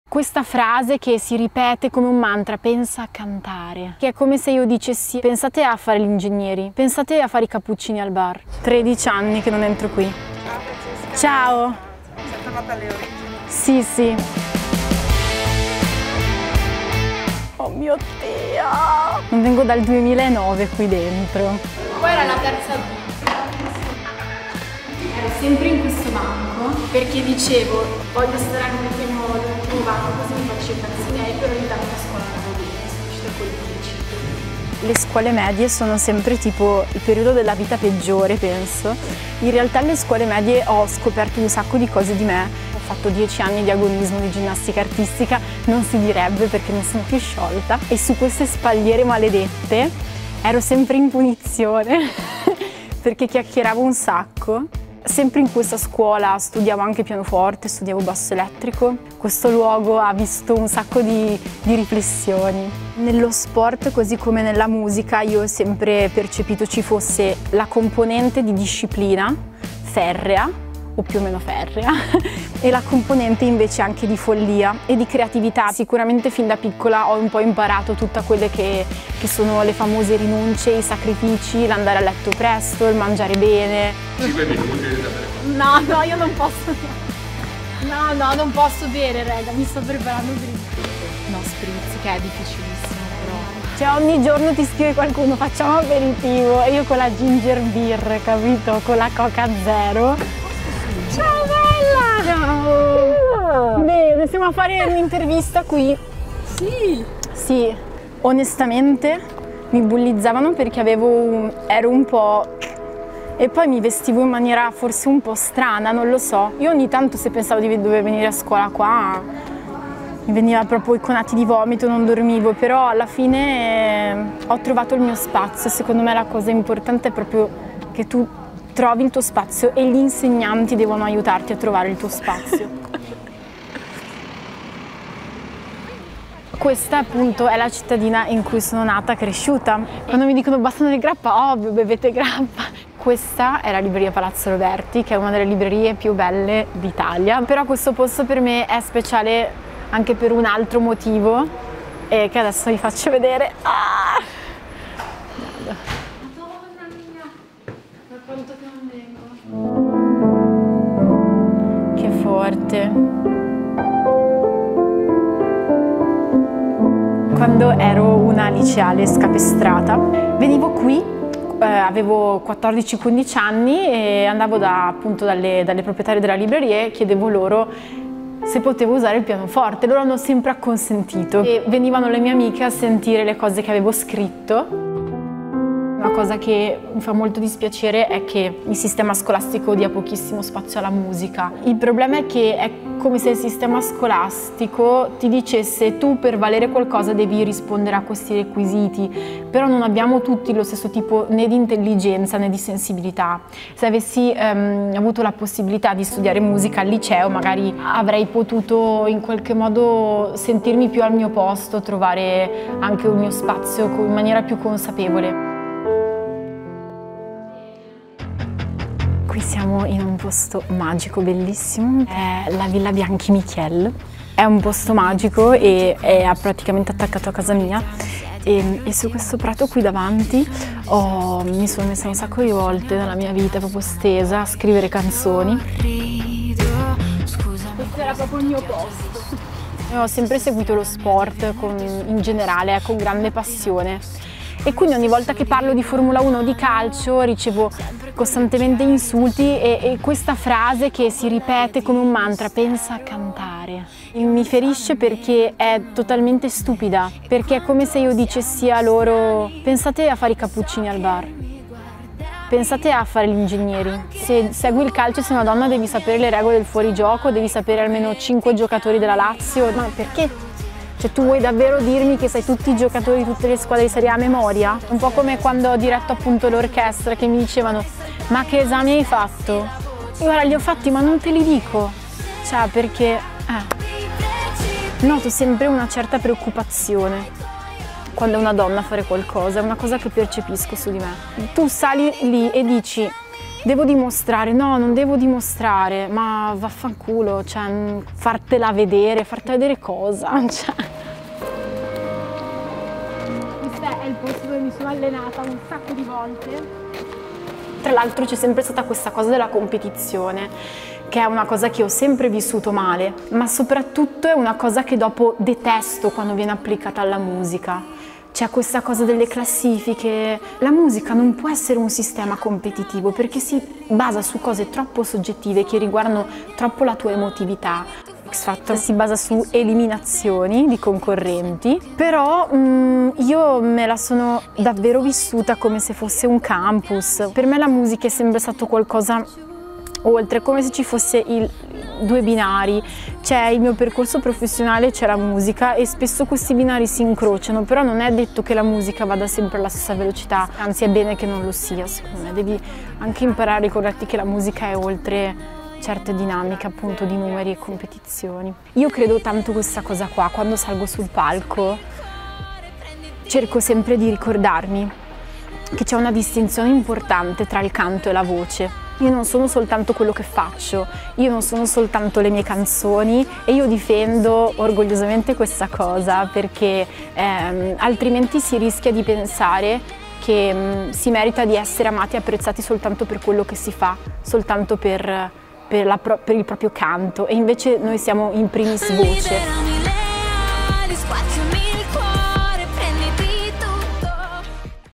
Alle origini di Francesca Michielin
Francesca Michielin non era mai tornata nella sua vecchia scuola media finché non le abbiamo chiesto di portarci lì. Tra un ricordo d'infanzia e l'altro, ha ripercorso con noi le tappe della sua vita e del suo amore per la musica tra le vie di Bassano del Grappa, città dov'è nata, cresciuta e abita tutt'oggi.